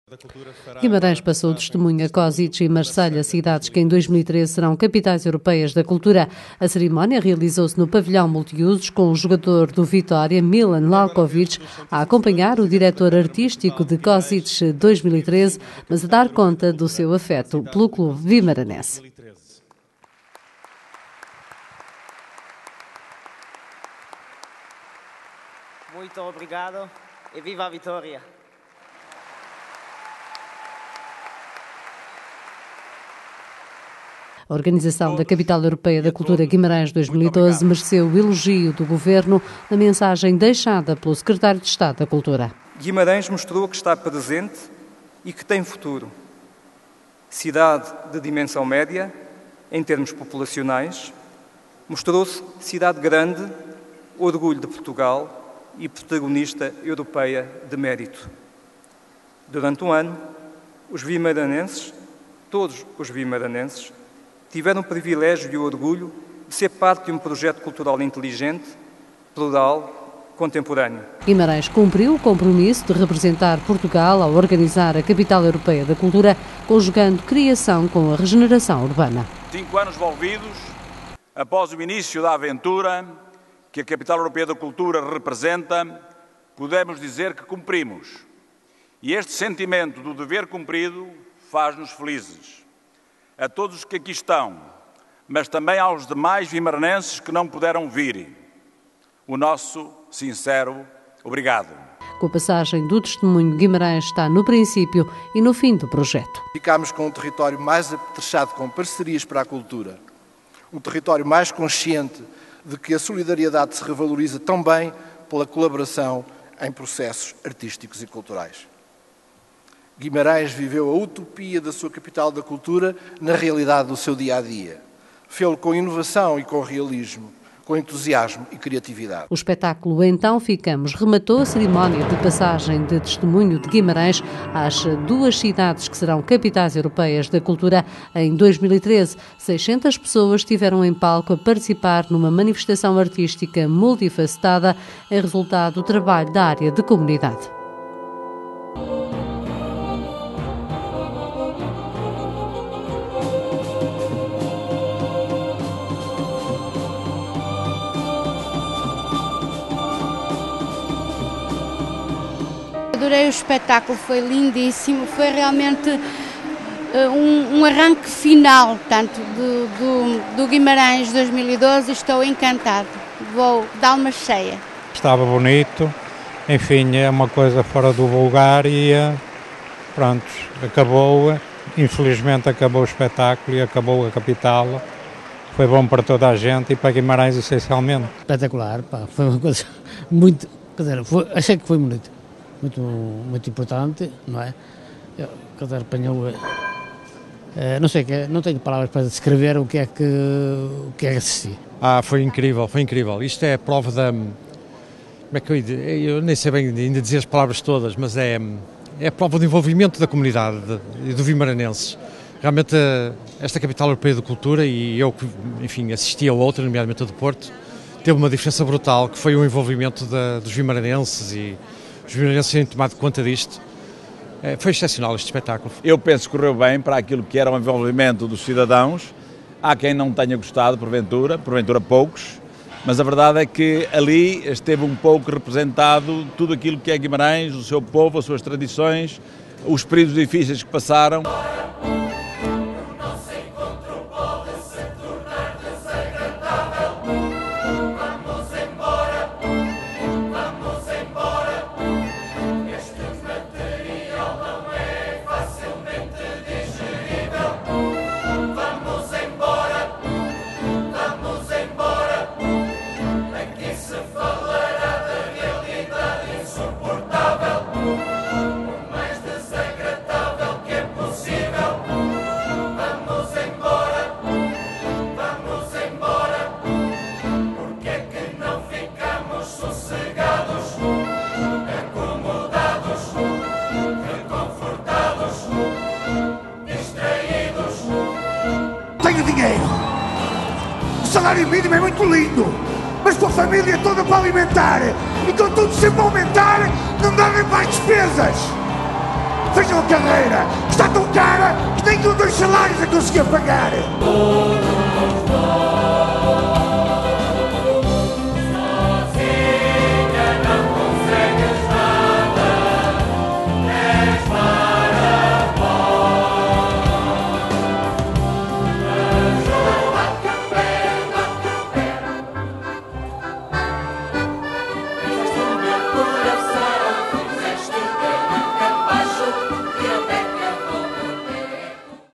«Então ficamos...» espectáculo de encerramento da Guimarães 2012
| Guimarães passou o testemunho a Kosice e Marselha, cidades que em 2013 serão Capitais Europeias da Cultura na Eslováquia e em França.
Na cerimónia realizada ontem à noite no Pavilhão Multiusos de Guimarães, o Secretário de Estado da Cultura deixou o elogio do Governo à organização da Capital Europeia da Cultura «Guimarães 2012».  "Guimarães deixou claro que não é só passado. Está presente e tem futuro. Mostrou ser Cidade grande e orgulho de Portugal", afirmou Jorge Barreto Xaxier, ao congratular-se com o êxito da Capital Europeia da Cultura.

Em tempo de contenção financeira, o governante apontou o "valor da presença, anunciando que o exemplo de Guimarães 2012 será reflectido num fórum a realizar no próximo ano.

Para o Presidente da Câmara, Guimarães cumpriu o compromisso de representar Portugal ao organizar a Capital Europeia da Cultura, conjugando a criação com a renegeração urbana. "Cumprimos porque estreitamos as relações entre a cultura e a regeneração social. Cumprimos porque todos fazemos parte desta extraordinário aventura de reinvenção deste lugar que habitamos", assinalou António Magalhães.

Com a passagem de testemunho, Guimarães "está no princípio e no fim do projecto". O Presidente da Fundação Cidade de Guimarães referiu que a Cidade "está na transição que agora começa. Com um espaço público enriquecido, vivido de forma intensa. Com a abertura renovada à pluralidade cultural e artística e ao cosmopolitismo".

O espectáculo "Então ficamos" rematou a cerimónia de passagem de testemunho de Guimarães às cidades de Kosice e Marselho. 600 pessoas estiveram em palco a participar numa manifestação artística  multifacetada, em resultado do trabalho da área de Comunidade.